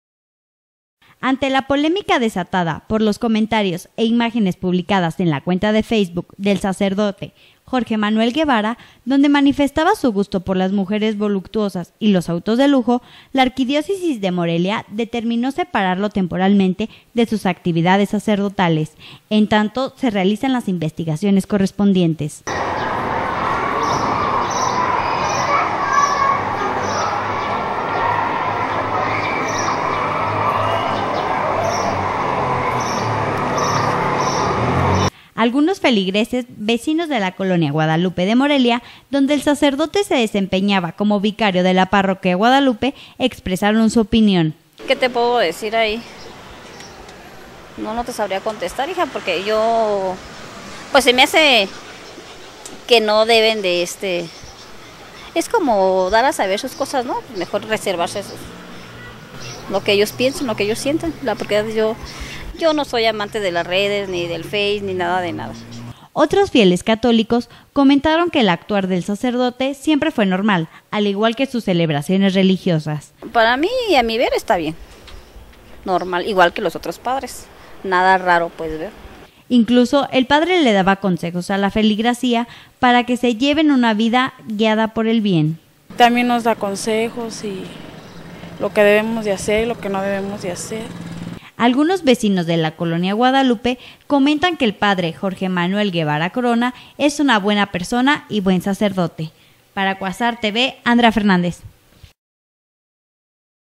Suspenden al sacerdote que gusta de mujeres y autos
Ante la polémica desatada por los comentarios e imágenes publicadas en redes sociales, del sacerdote Manuel Guevara donde manifestaba su gusto por las mujeres voluptuosas y los autos de lujo, la arquidiócesis de Morelia determinó separarlo temporalmente de sus actividades sacerdotales, en tanto se realizan las investigaciones correspondientes...